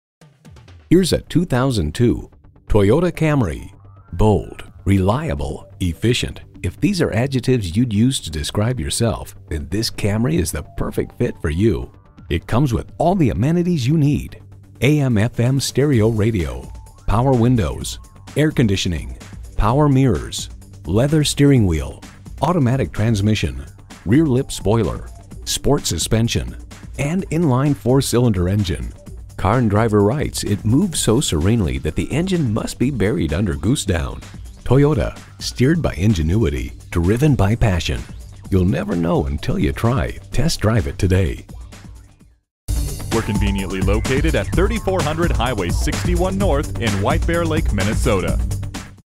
Used 2002 Toyota Camry Saint Paul White-Bear-Lake, MN W99012T - SOLD
Year: 2002
Make: Toyota
Model: Camry
Trim: SE
Engine: 4 Cylinder Engine
Transmission: Automatic
Color: Desert Sand Mica
Interior: Stone
Mileage: 166907
Stock #: W99012T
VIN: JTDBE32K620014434

Address:
3400 Highway 61 North